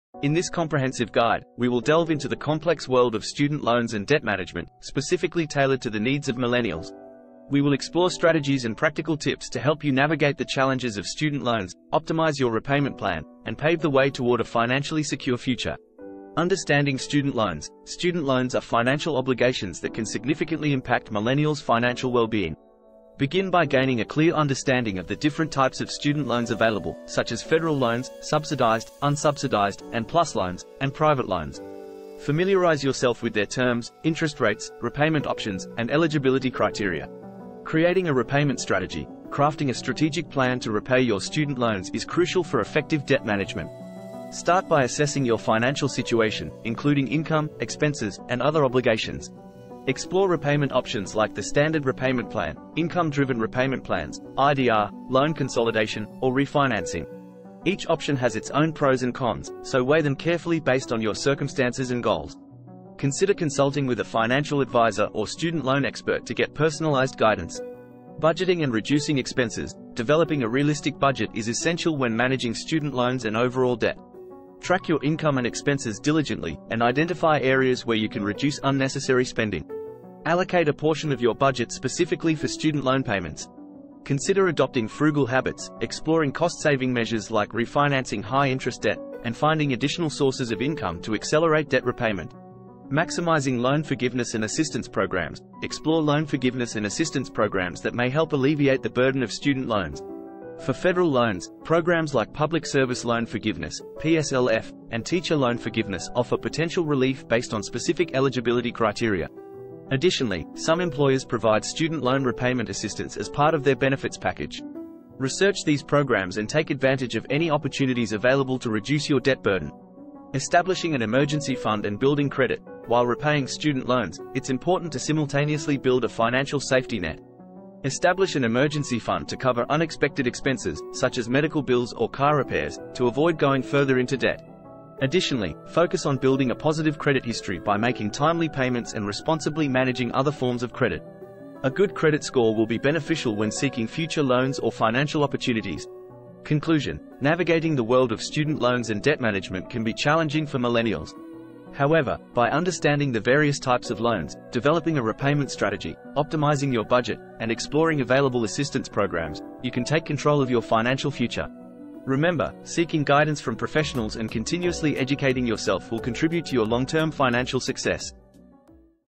🎒📚💰Mastering Student Loans and Debt Management🔥
In this comprehensive guide, we provide essential tips and strategies for millennials to effectively manage student loans and debt. Learn about different types of student loans, repayment options, budgeting techniques, and loan forgiveness programs. Take control of your financial future and pave the way towards a debt-free life. Watch now!

Source:Chatgpt